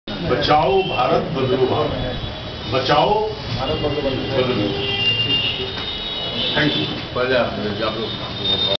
JNU student leaders will be in kashi
JNU student leaders will be in kashi  on long march started from kanyakumari